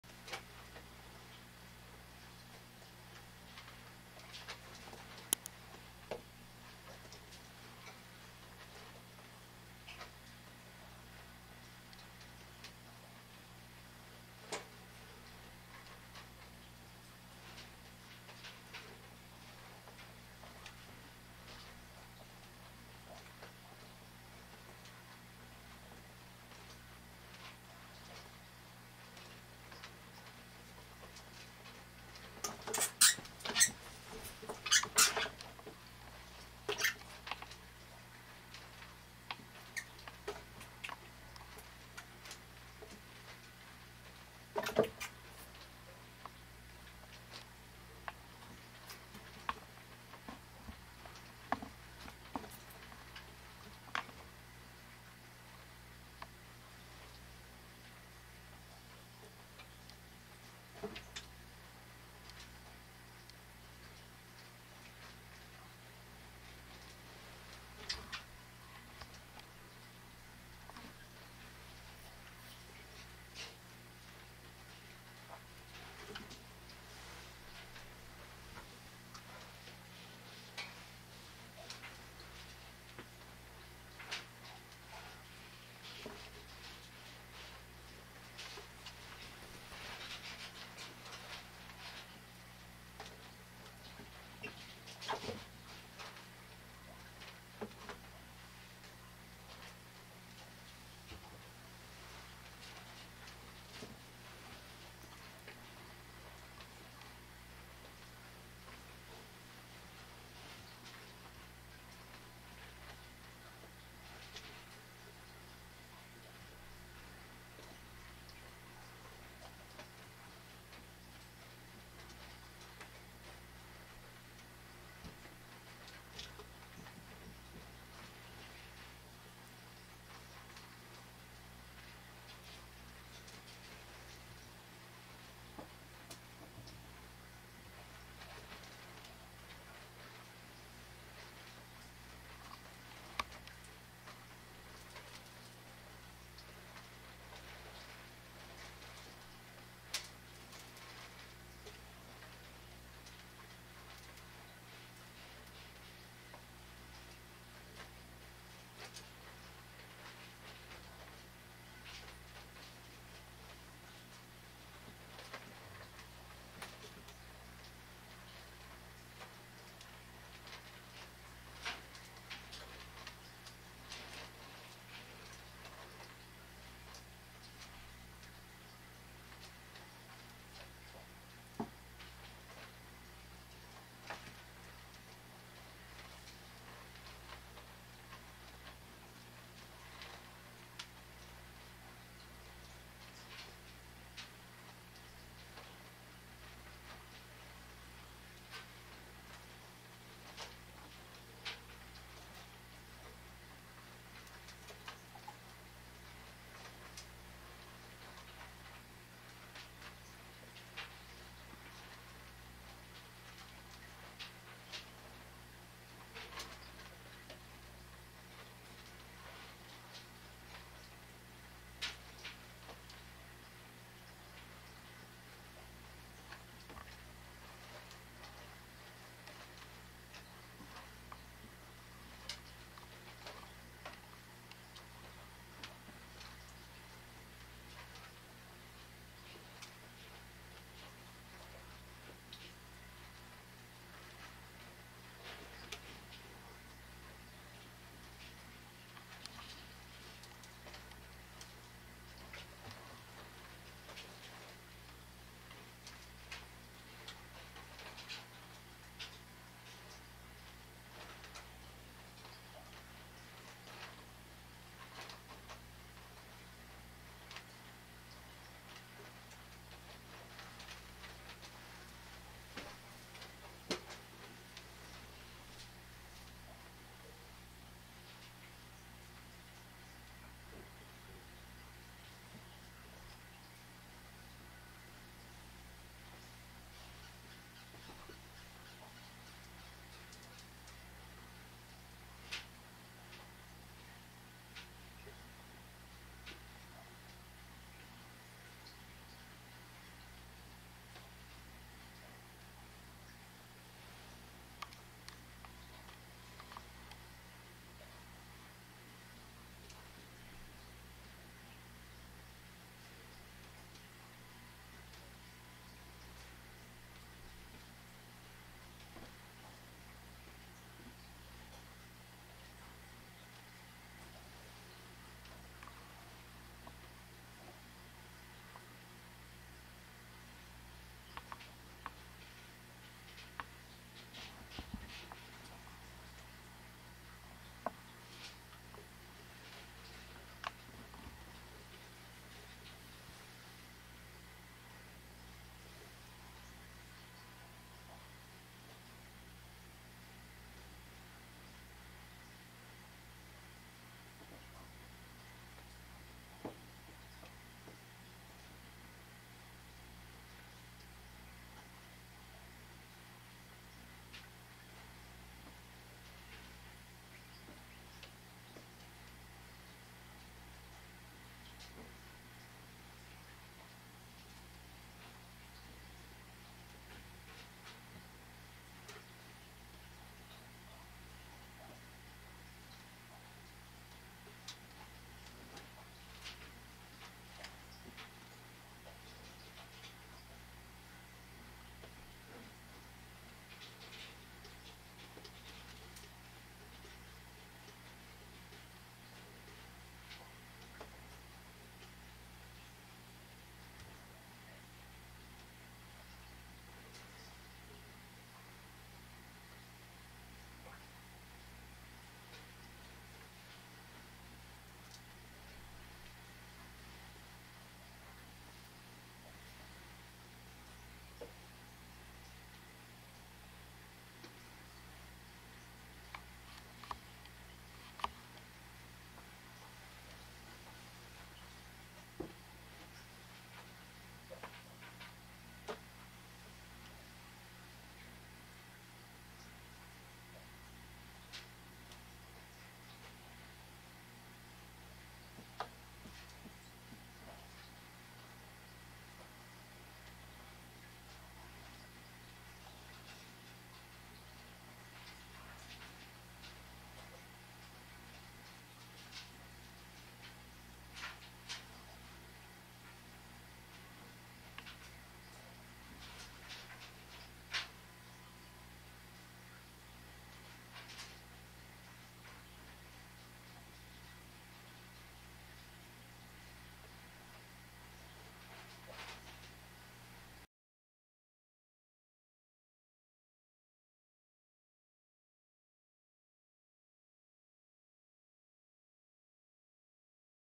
BullFrog Eats A Hamster - Warning Live Feeding
This is a video documenting the live feeding habits and responses of The Giant African Bullfrog .